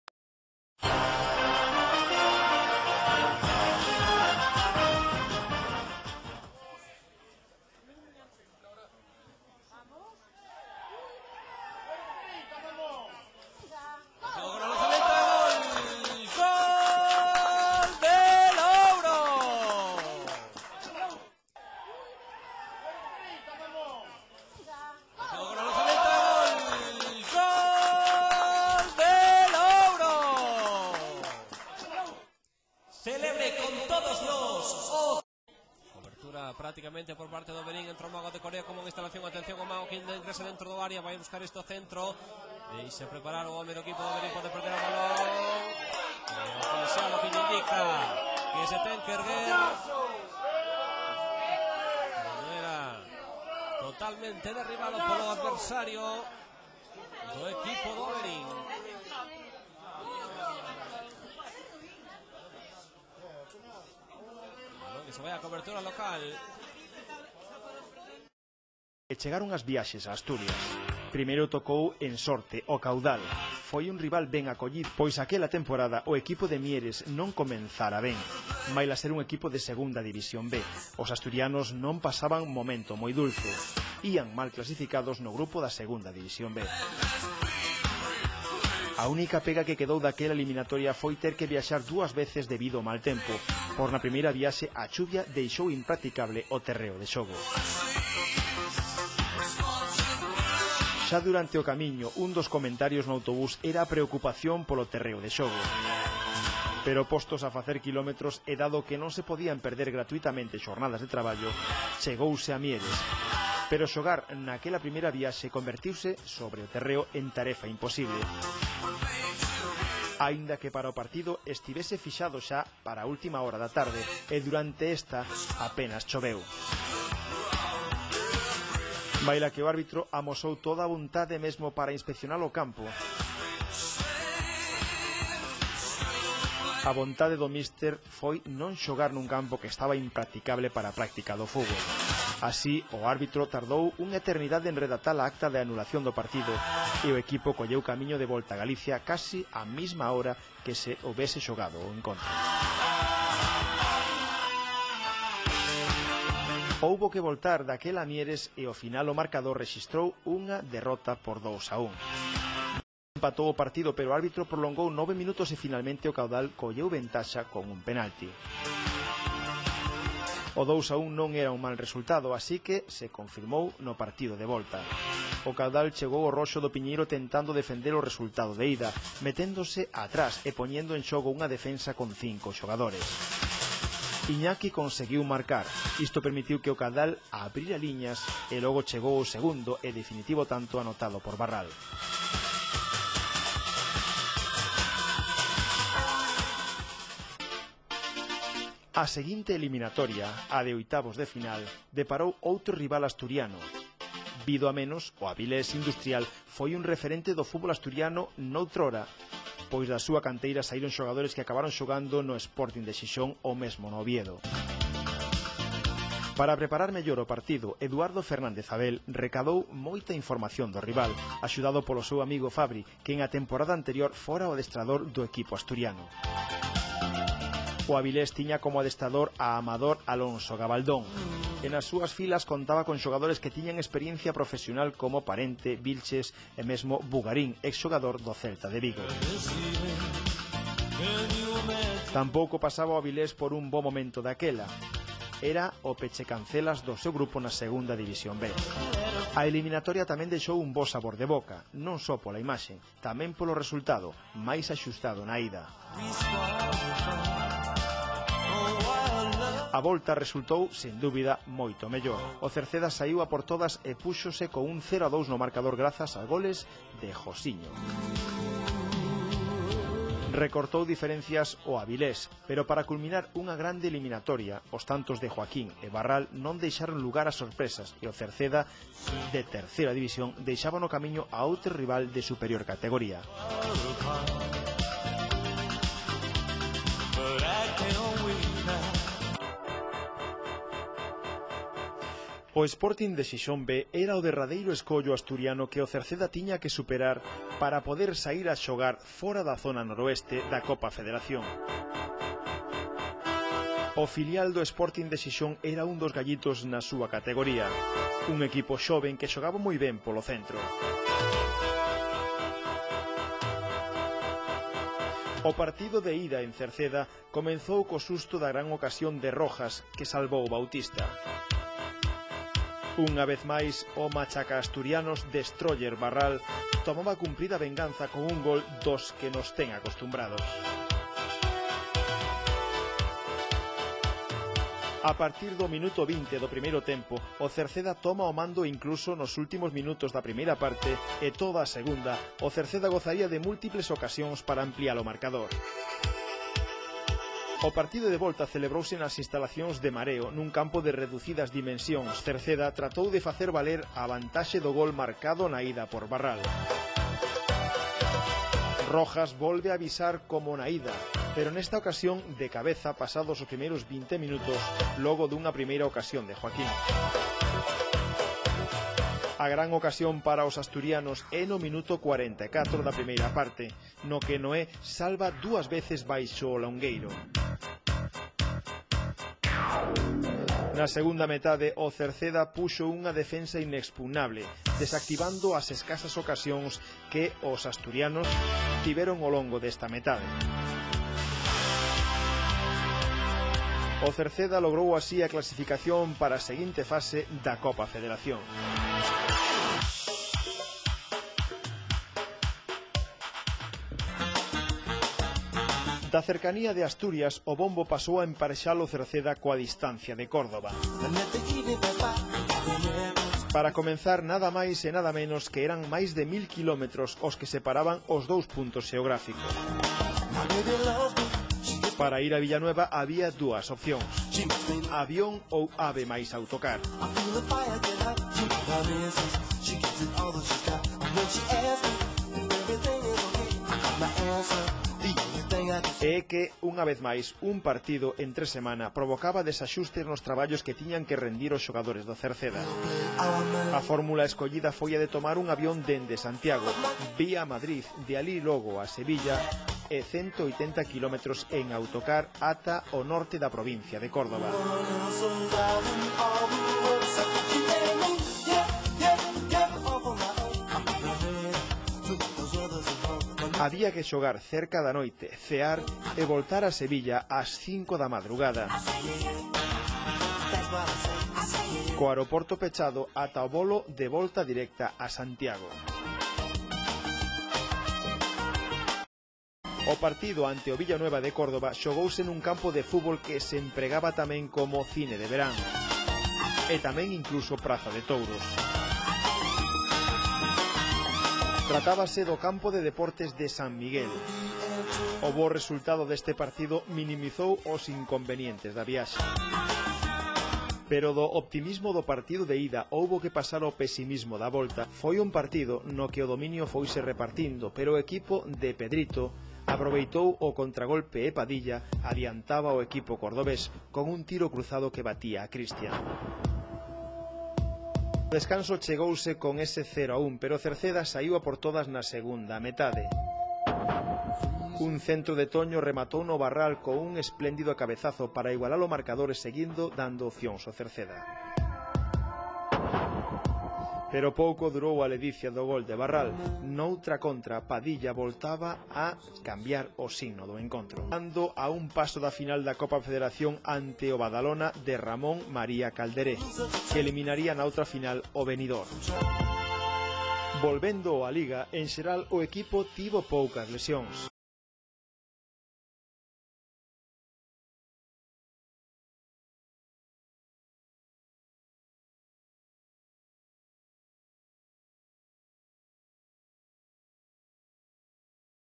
CERCEDA-VERIN (6-0)- TELEVISION CAMARIÑAS
Imaxes cedidas por Tv. Cerceda (Anxo), con imaxes de Juanillo dando un pase de gol e un penalti ao xogador
camariñán que non foi pitado polo árbitro do encontro. Tamén incluimos imaxes históricas da participación
do Cerceda na Copa Federación e cando tivo que viaxar a terras cordobesas.